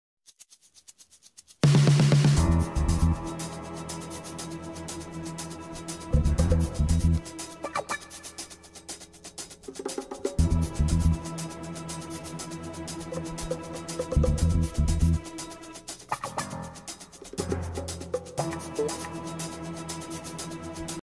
Mlp random ships : 11
The magic was supposed to be a light green but it turned out as this disgusting yellow on camera . He's not a failure though because the pose turned out well despite it being experimental.